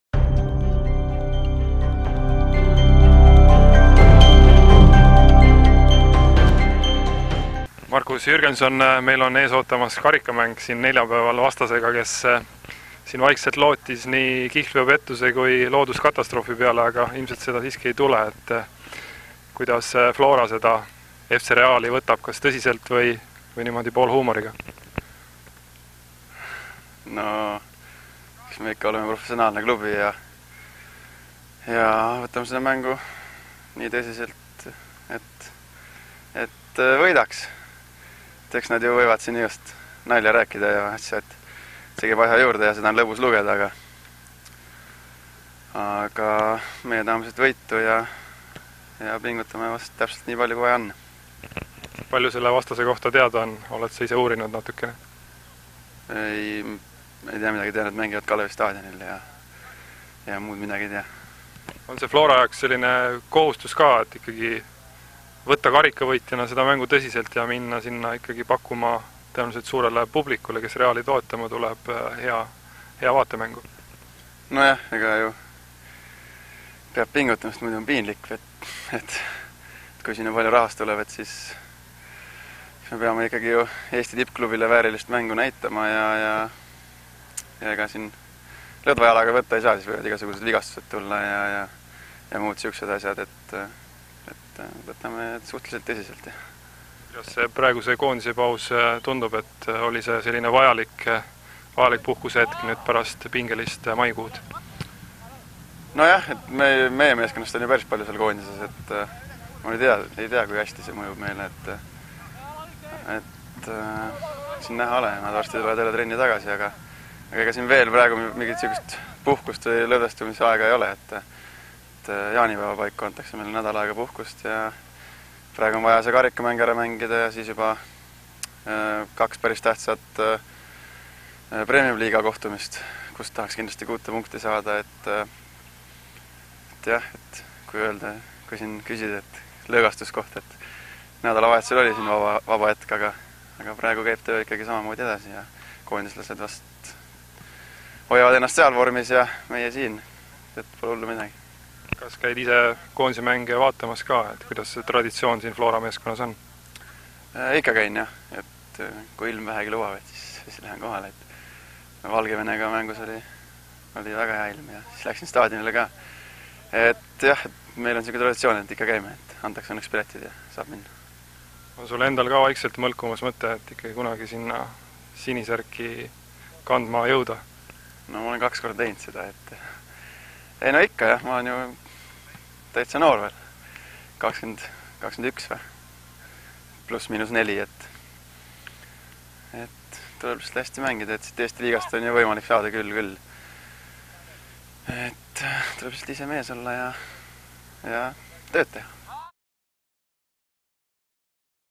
TV FLORA: Markus Jürgenson päev enne karikamängu FC Reaaliga
Eesti karikavõitja FC Flora alustab uut karikahooaega neljapäeval, mil 1/64 kohtutakse võõrsil Tallinna FC Reaaliga.